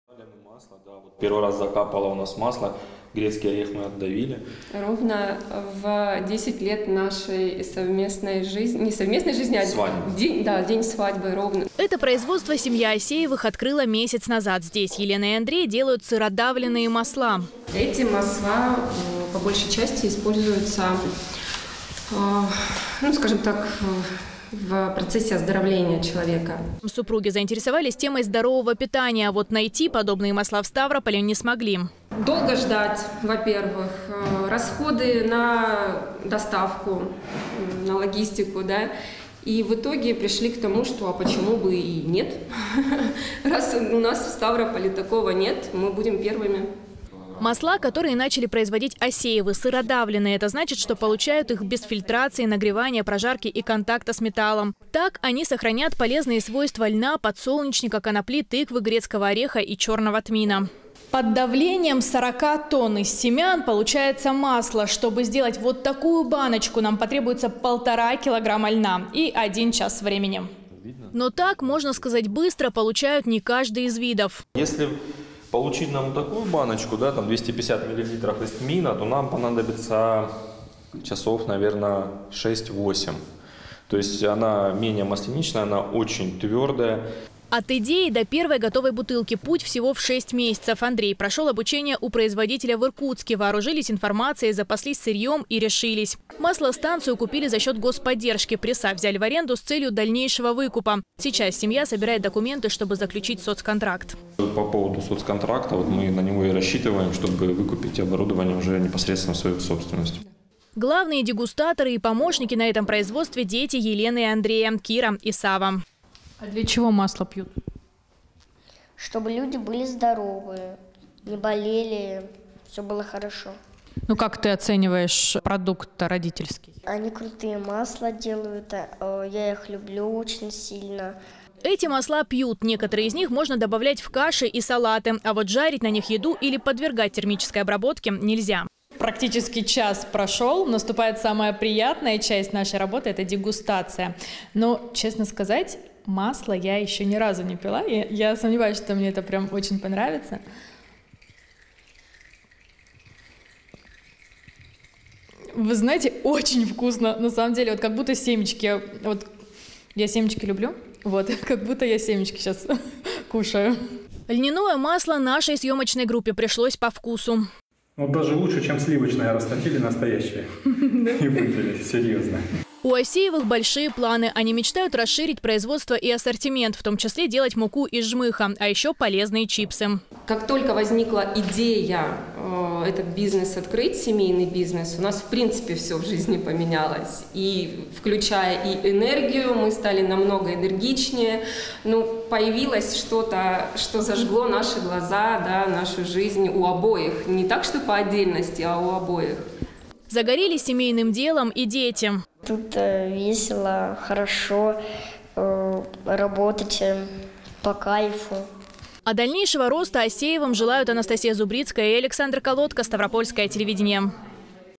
Супруги из Ставрополя открыли производство сыродавленных масел
Производство работает 3 недели

Елена и Андрей Асеевы заинтересовались правильным питанием. Однако, найти нужные масла в Ставрополе они не смогли. Поэтому решили делать их сами.

«Долго ждать, во-первых, расходы на доставку, на логистику, да и в итоге пришли к тому, а почему бы и нет? раз у нас в Ставрополе такого нет, мы будем первыми» - рассказала корреспонденту Своего ТВ Елена Асеева.

Масла, которые начали производить Асеевы - сыродавленные. Это значит, что получают их без фильтрации, нагревания, прожарки и контакта с металлом. Так они сохранят полезные свойства льна, подсолнечника, конопли, тыквы, грецкого ореха и чёрного тмина. От идеи до первой готовой бутылки путь - всего в 6 месяцев. Андрей прошёл обучение у производителя в Иркутске. Вооружились информацией, запаслись сырьём и решились. Маслостанцию купили за счёт господдержки. Пресса взяли в аренду с целью дальнейшего выкупа. Сейчас семья собирает документы, чтобы заключить соцконтракт.

«Вот как раз по поводу соцконтракта мы на него и рассчитываем, чтобы выкупить оборудование непосредственно в свою собственность» - отметил Андрей Асеев.